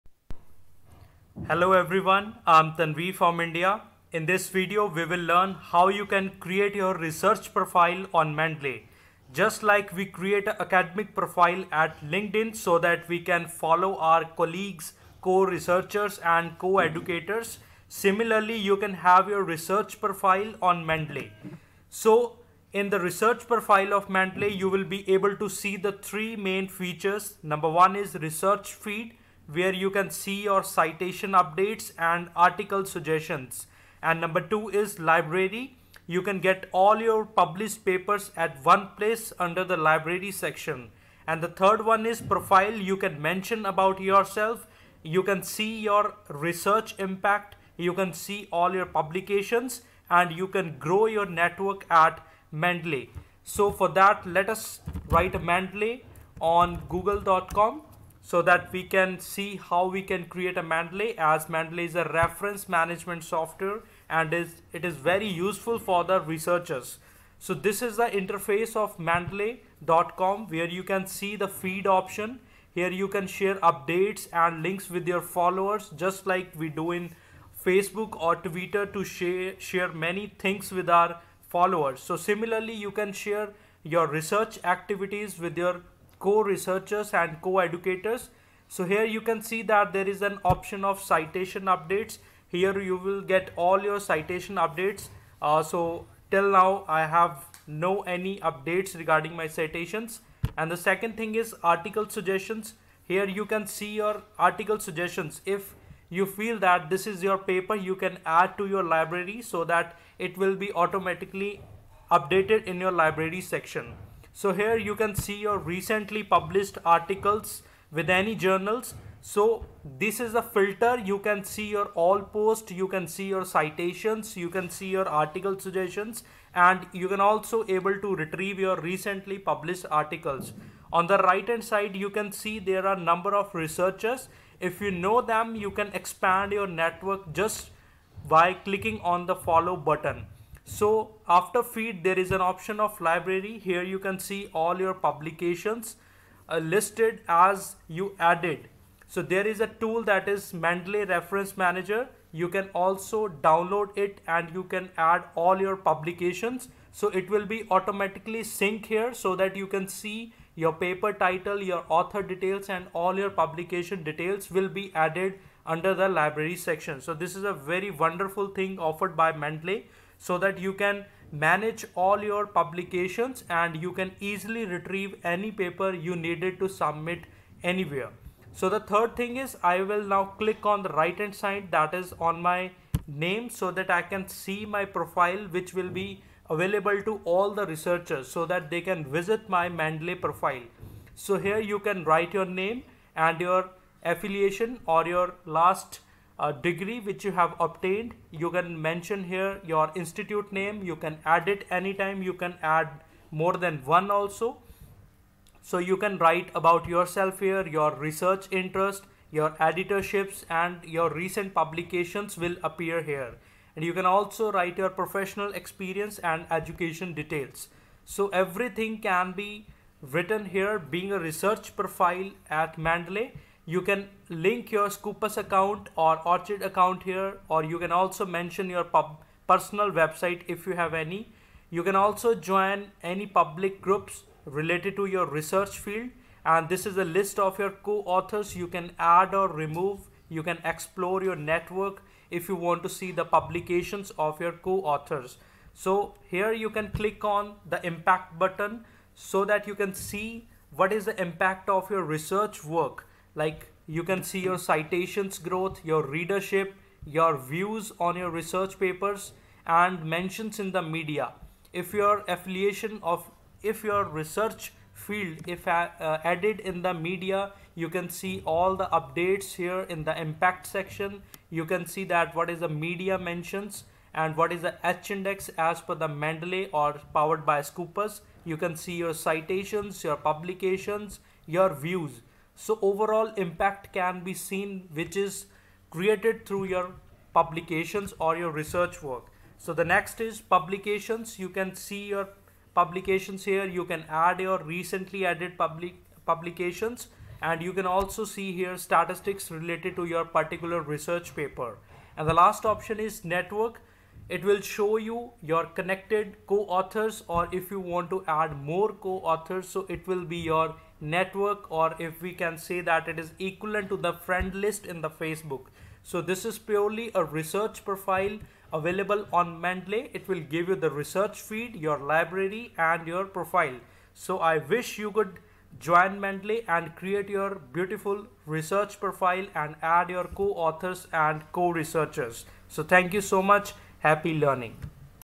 Create Research Profile on Mendeley | Learn Mendeley | Research Tool | Manage Publications
In this video, you will learn how you can create Research Profile on Mendeley.

On the platform of Mendeley you can create your research profile where you can add following items

1. You can see your research posts that can be shared with co-authors and co-researchers
2. You can get citation updates
3. You can see Article Suggestions
4. You can manage your recent published articles

Under Library section, you can add and manage your all published papers.

Profile on Mendeley will have following parameters to add:

- About yourself
- Research Interest
- Editorship
- Recent Publications
- Co-authors
- Citations
- Readers
- Views
- Mentions in Media
- H-index
- Network of Co-authors